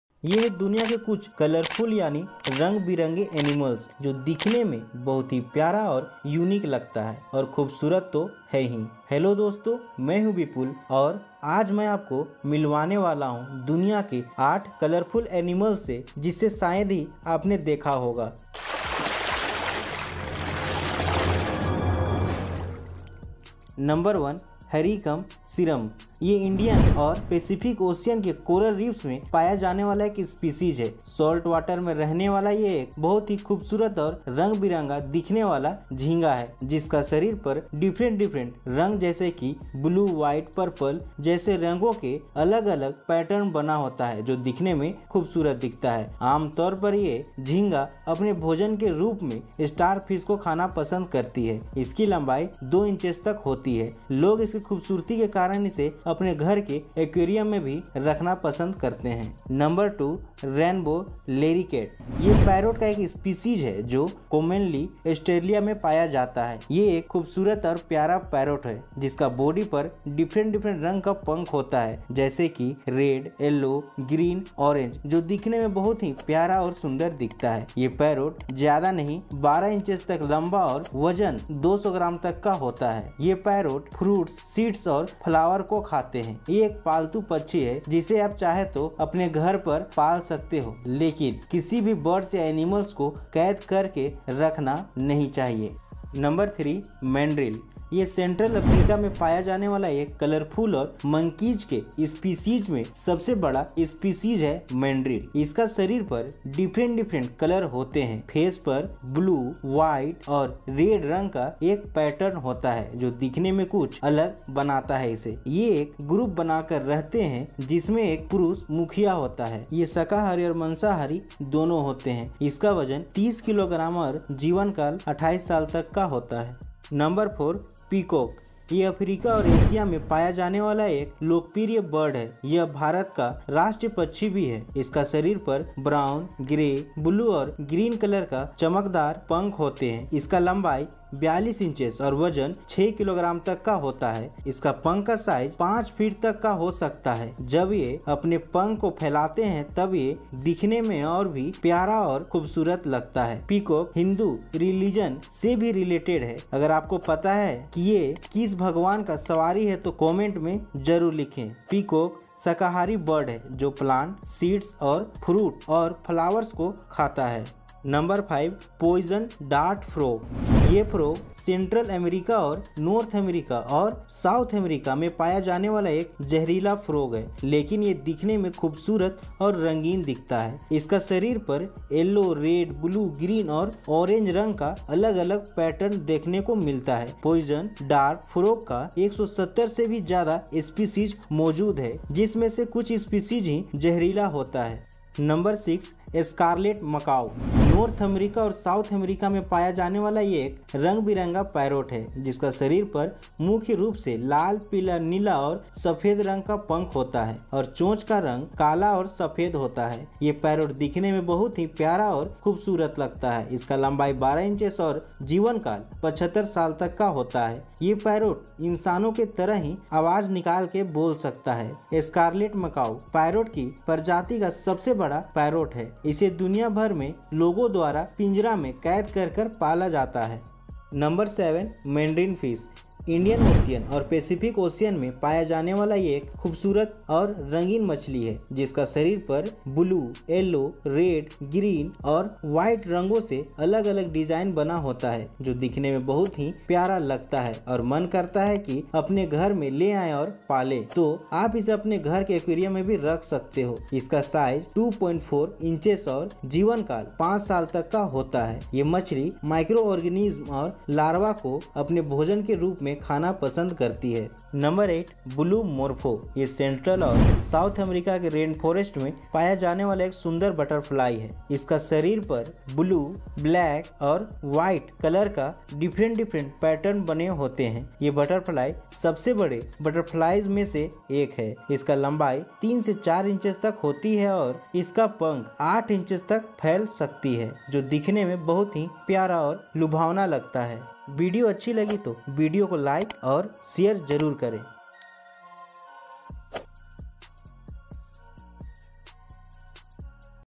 most colourful and beautiful animals in the world | दुनिया के सबसे रंगीन और खूबसूरत जानवर
Most colourful and beautiful animals in the world
Harlequin shrimp
Rainbow larikeet
Mandrill
Peacock
Poison dart frog
Mandarin fish
Blue morpho